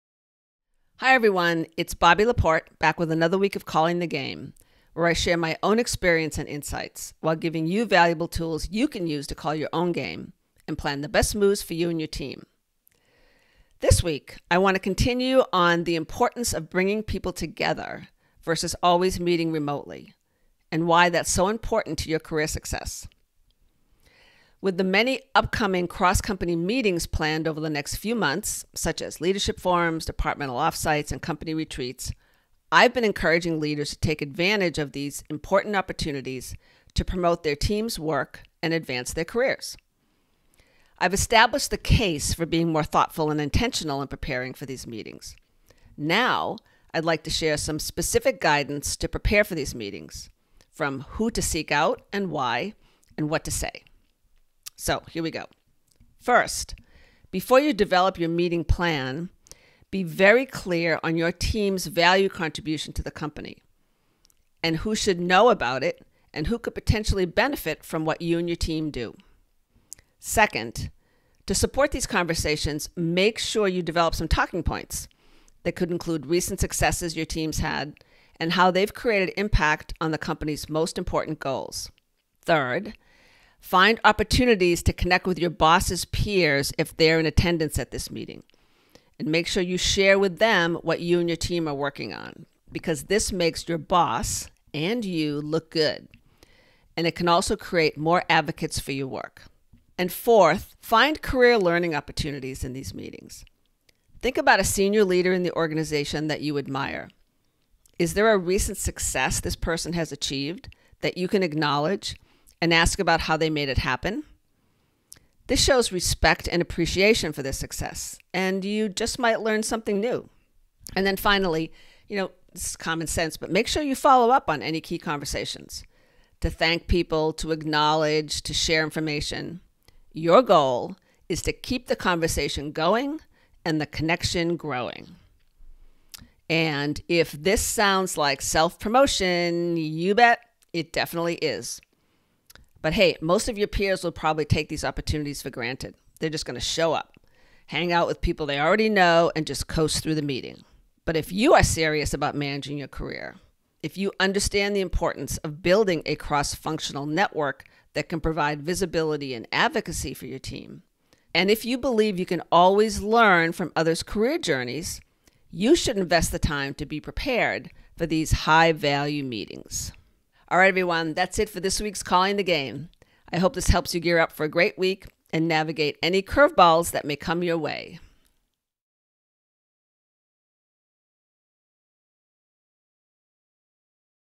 Prepare to Network Strategically at Cross-Company Events
Before you head off to your next leadership forum, offsite meeting, or company retreat, take 3 minutes to watch this week's "Calling the Game" video. You'll gain practical guidance on ways you can promote your team’s work, strengthen connections, and advance your own career.